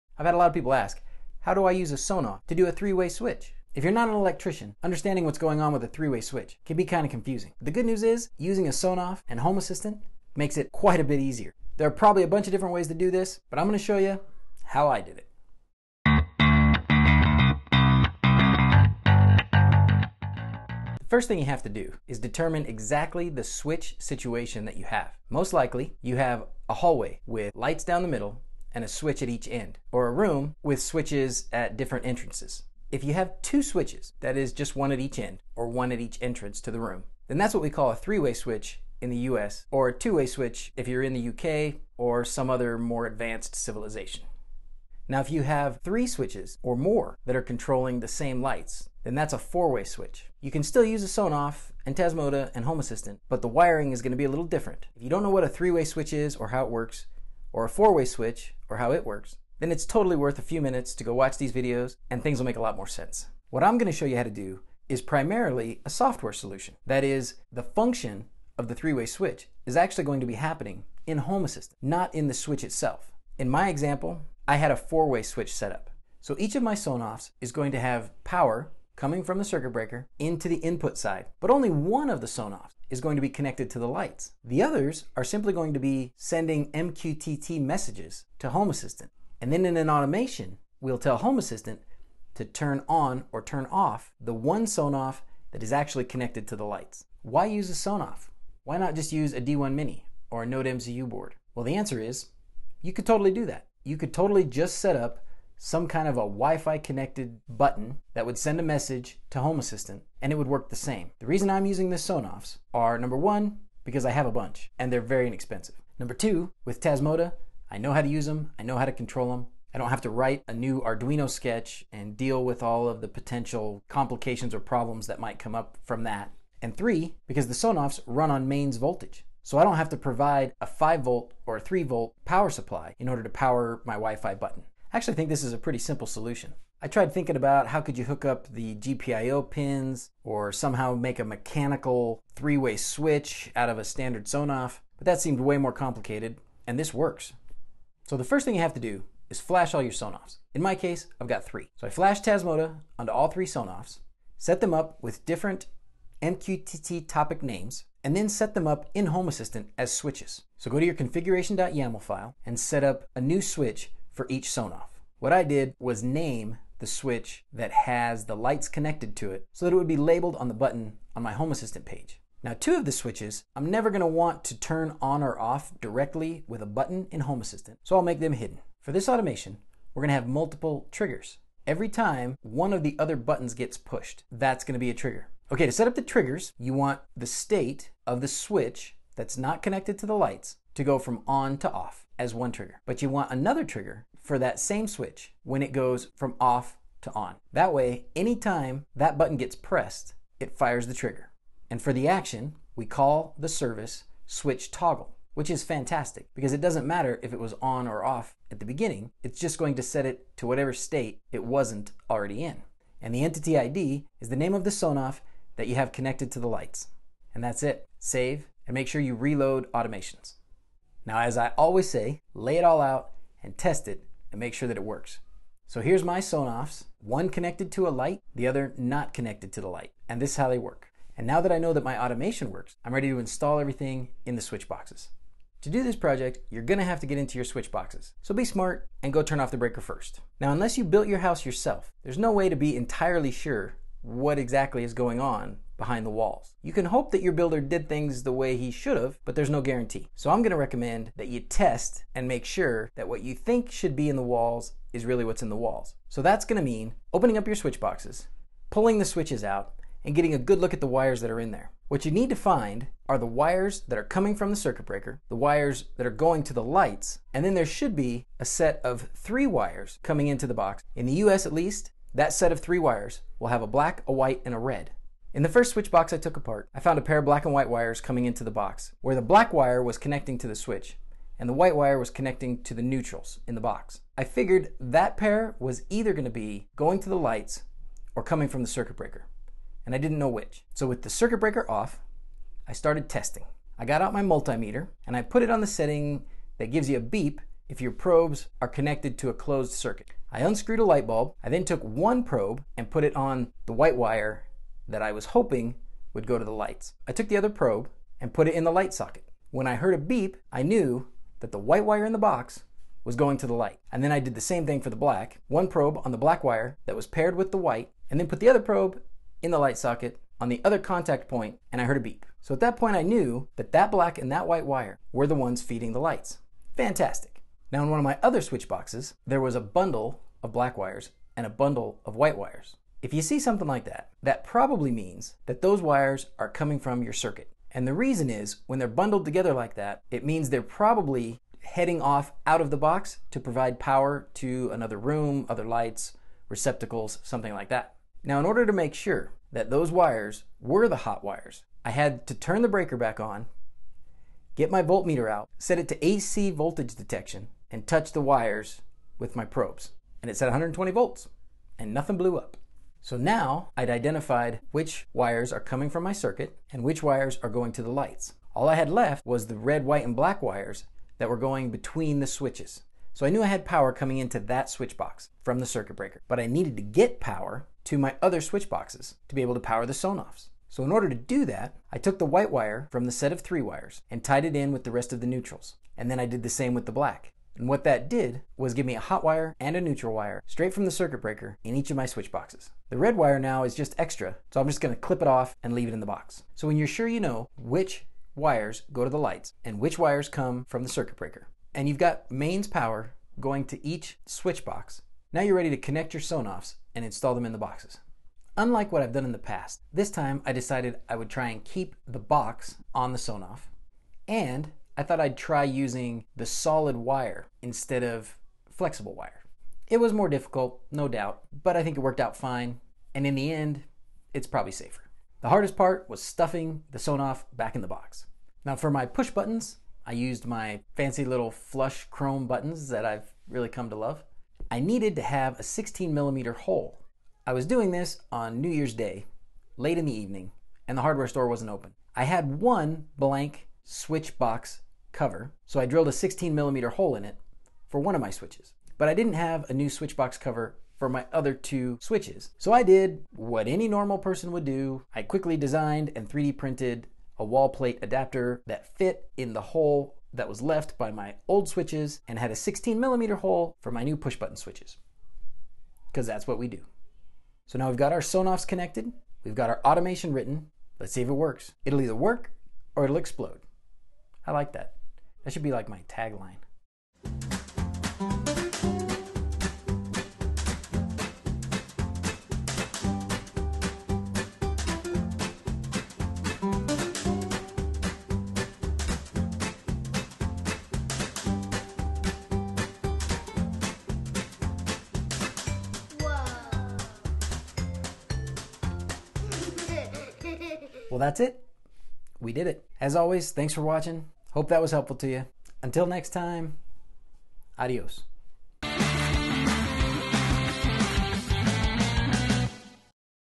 3-way Smart Switch! THE easy and cheap way!
Three way switching with Sonoff/Tasmota/Home Assistant
This has been by far the most requested topic, so here you go!

I found a couple short videos that I think explain 3 and 4 way switches pretty well.

You need one Sonoff for each switch you want to replace.

One Sonoff will be connected to the lights. The others will just be mqtt buttons.
The real 3-way switch work is done with an automation in Home Assistant.
If you don’t have Home Assistant yet.  Now’s the time!

Home Assistant Switch set-up:
switch:
  - platform: mqtt
    name: "Light Connected"
    command_topic: "cmnd/sonoff/power"
    state_topic: "stat/sonoff/POWER"
    qos: 1
    payload_on: "ON"
    payload_off: "OFF"
    retain: true
  - platform: mqtt
    name: "Sonoff2"
    command_topic: "cmnd/sonoff2/power"
    state_topic: "stat/sonoff2/POWER"
    qos: 1
    payload_on: "ON"
    payload_off: "OFF"
    hidden: true
    retain: true

Home Assistant Automation: (I cut this from my Automation.yaml)
- action:
  - data:
      entity_id: switch.upstairs_hall
    service: switch.toggle
  alias: Multi Switch Hall Lights
  condition: []
  id: '0'
  trigger:
  - entity_id: switch.sonoff_11
    from: 'on'
    platform: state
    to: 'off'
  - entity_id: switch.sonoff_11
    from: 'off'
    platform: state
    to: 'on'